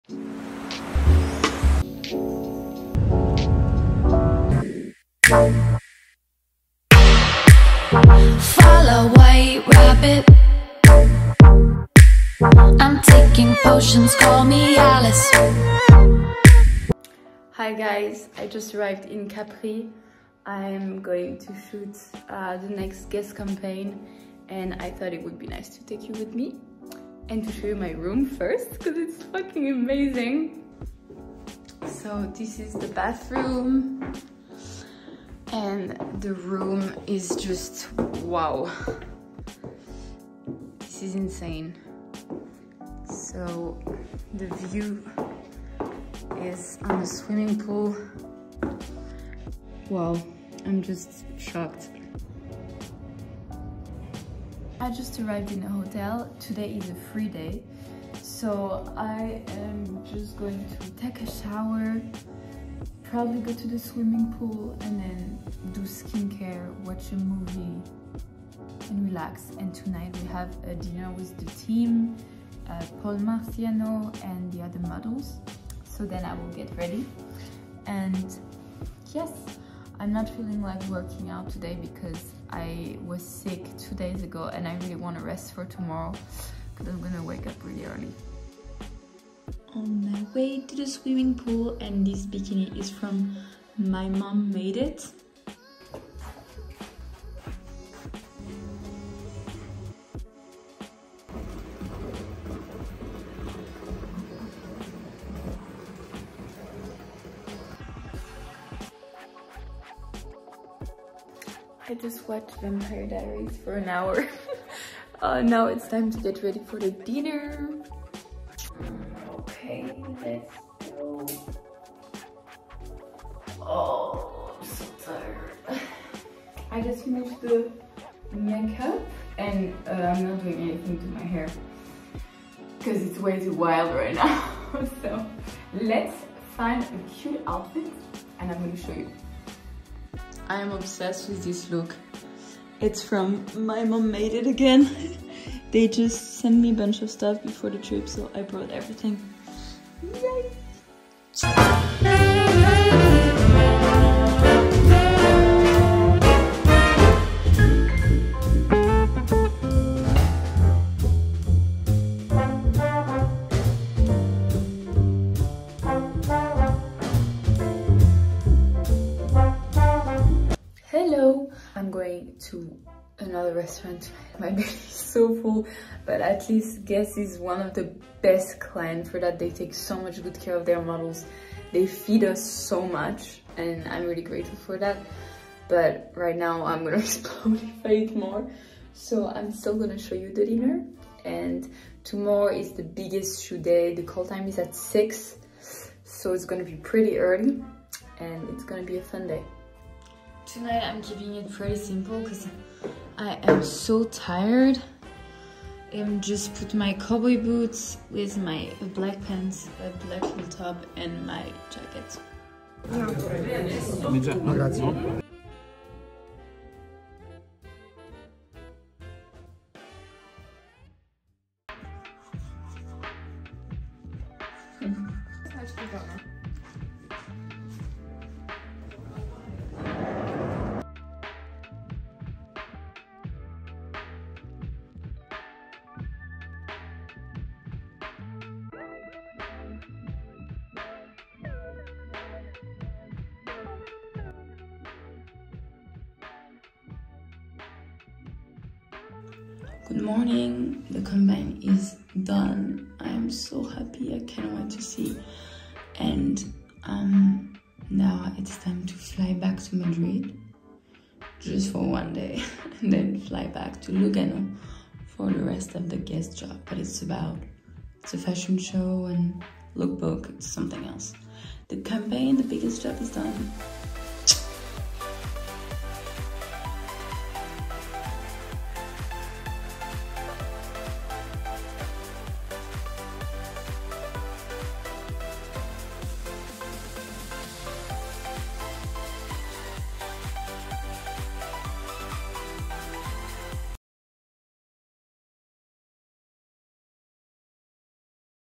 Guess campaign in Capri “The backstage” || Julie Tuzet
Hi everyone !! Hope you will enjoy this spontaneous backstage vlog ! I am so grateful for this campaign 😍
Big thanks to the entire team and to Paul Marciano for scouting me 🙏

INSTA TAGS
Models : @julie.tuzet

Photographers : @vicoolyasaida
Créative director : @paulmarciano